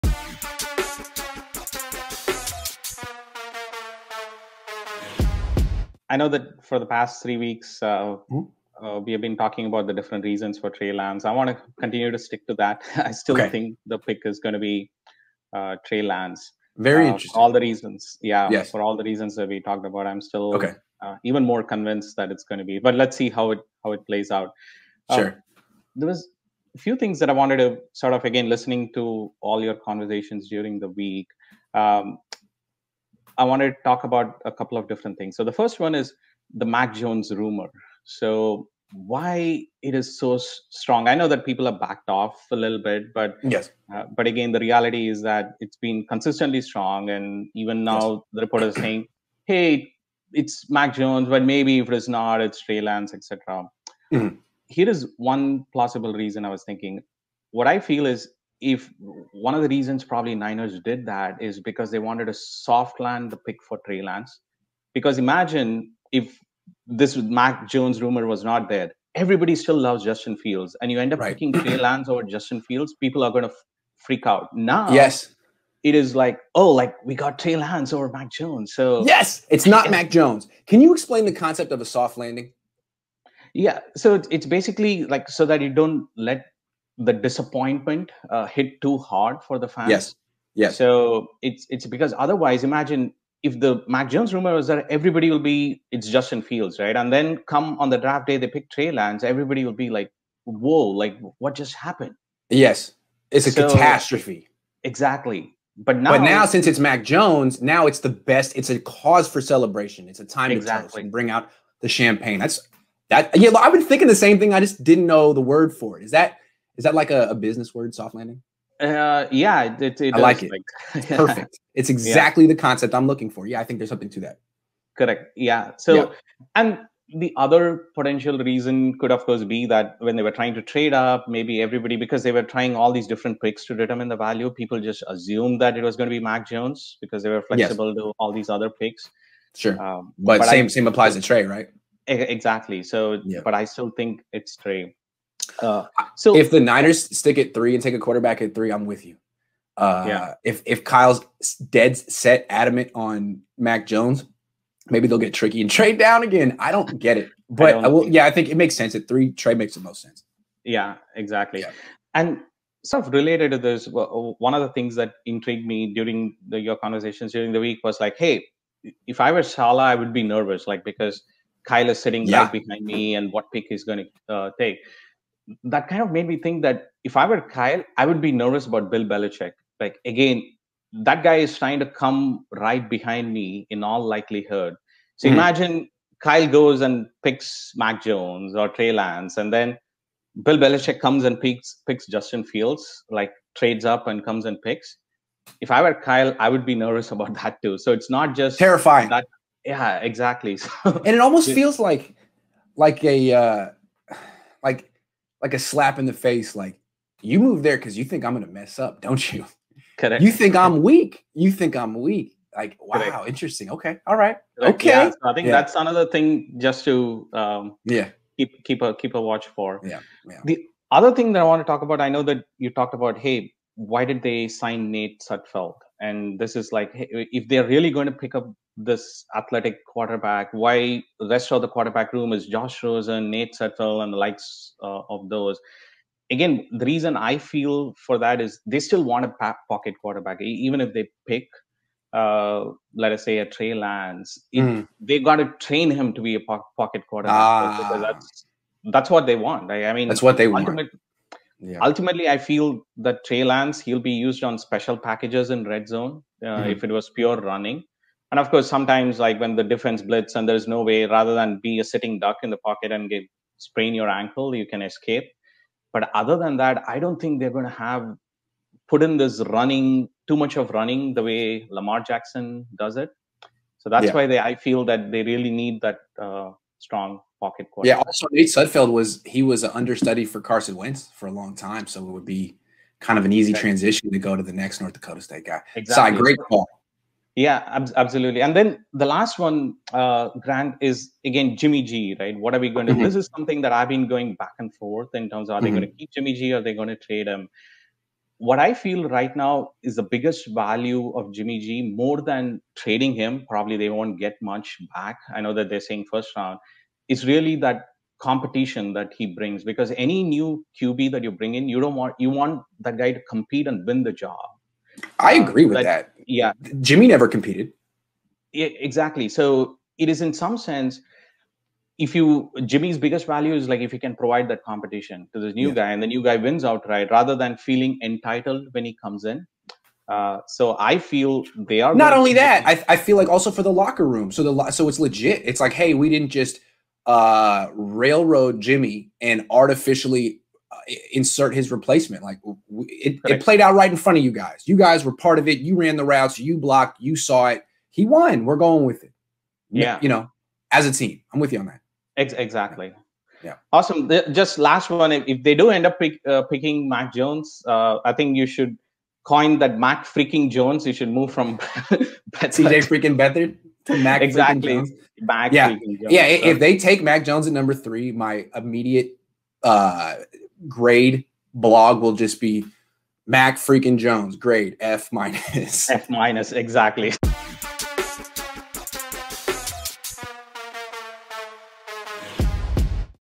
Are the 49ers “Soft-Landing” Their Pick of Trey Lance?
Grant Cohn and Sai Natarajan discuss whether the San Francisco 49ers are "soft-landing" their pick of Trey Lance.